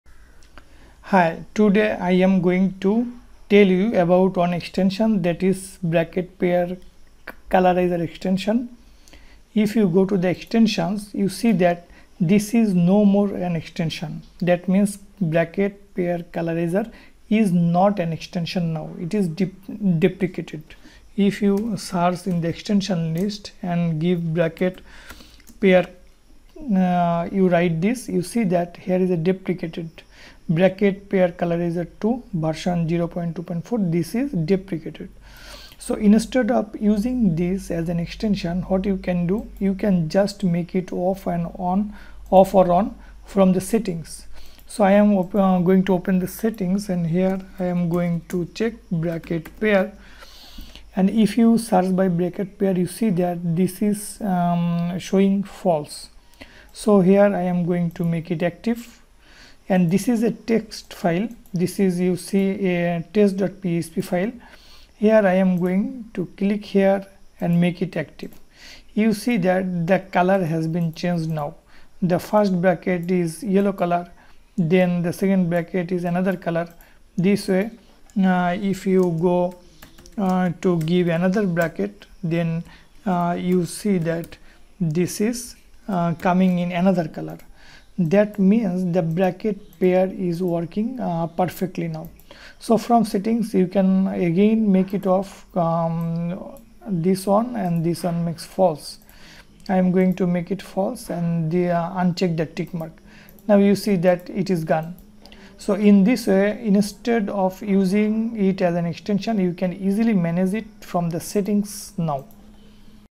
10. Bracket Pair Colorizer - This is not extension anymore | VS Code Editor
In this tutorial, I have shown how to work bracket pair colorizer in VS code editor. I hope, you will be benefitted from this tutorial.

If you want to see video tutorials on real life complete projects, get those on Udemy: